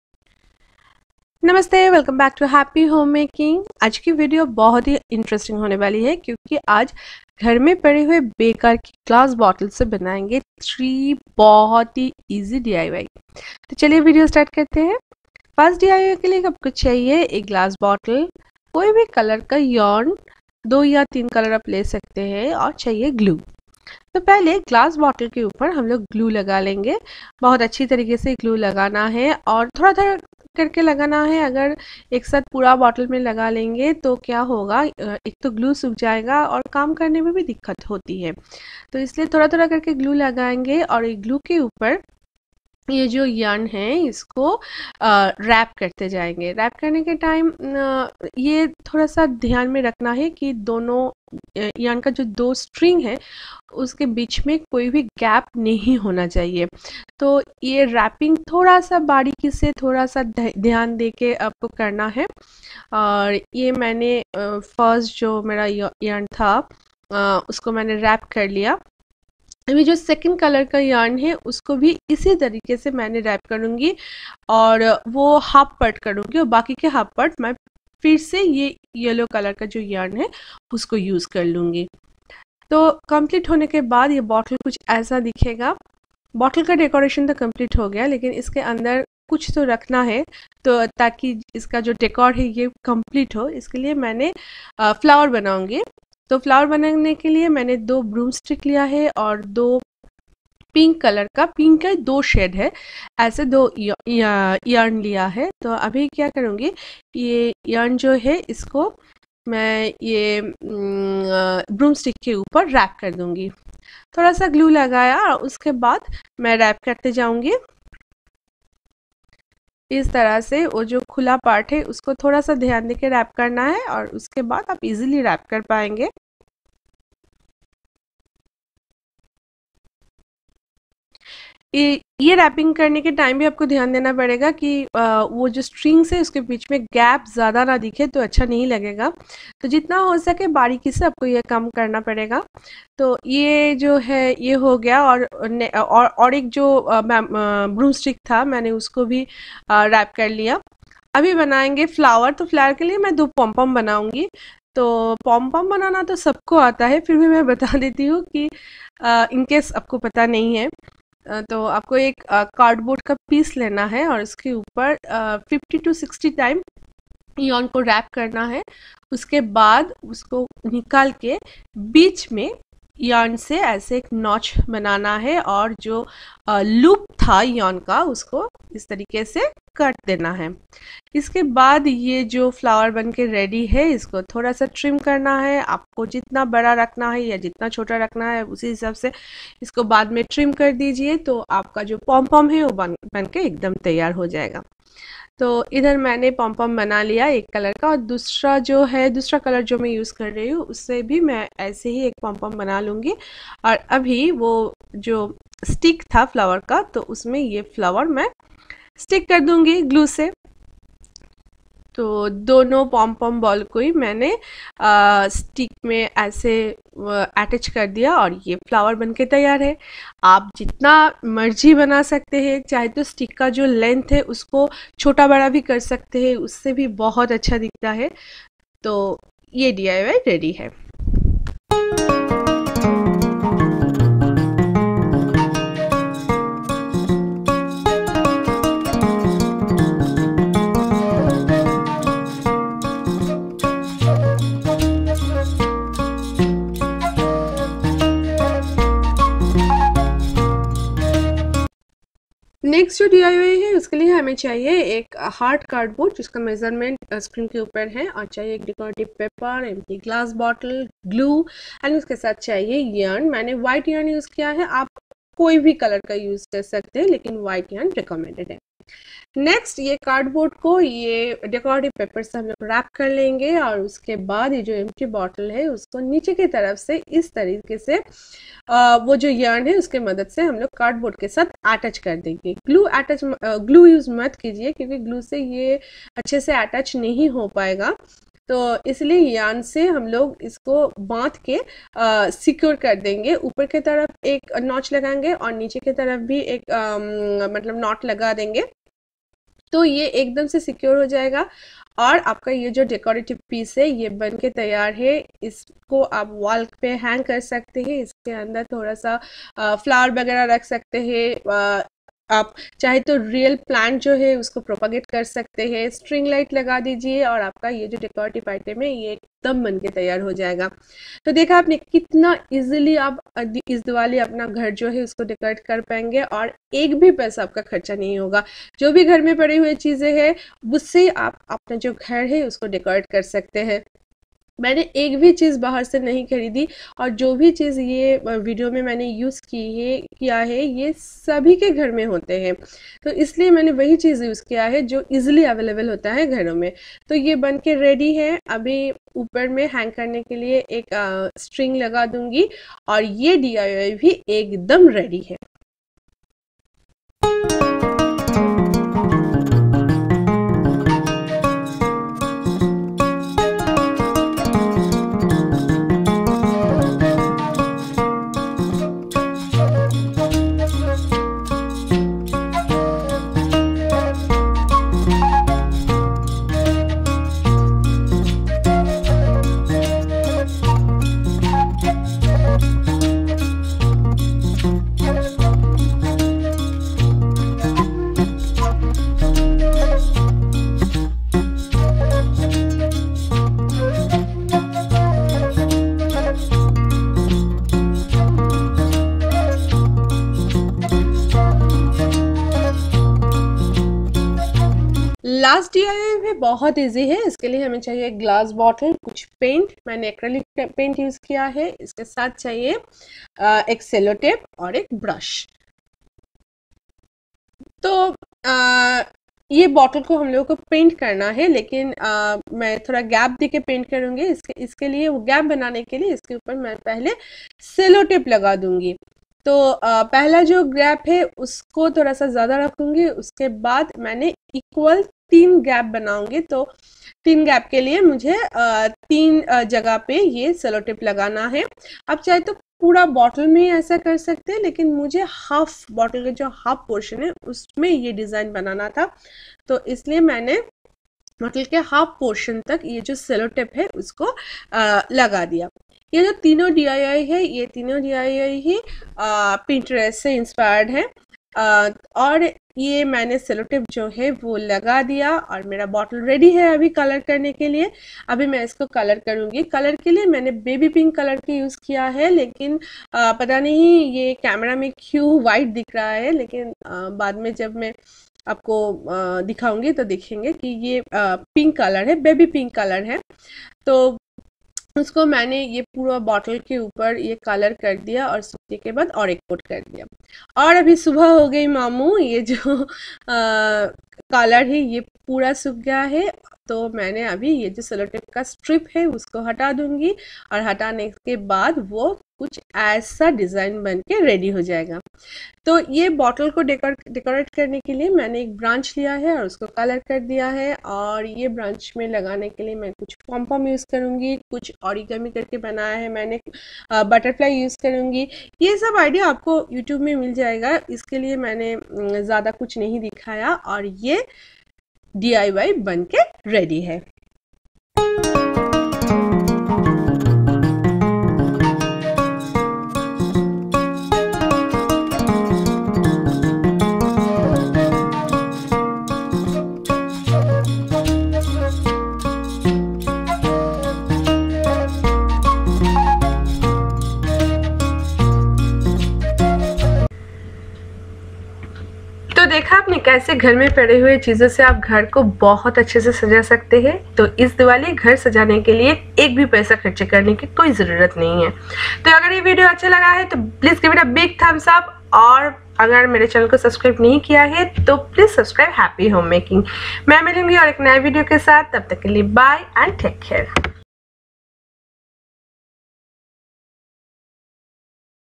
Empty Glass Bottle décor | DIY | Best out of waste | Trash to treasure - Recycle | Diwali Decoration
In today's Video I am Going to Share 3 easy DIY with empty glass bottle. All the materials that are used  video are easily available at home. Hope you will enjoy the video.

Love
Amrita
Bottle decor, Empty bottle diy, Craft, artkala, easy bottle art, old glass bottle reuse, repurpose waste, recycle waste,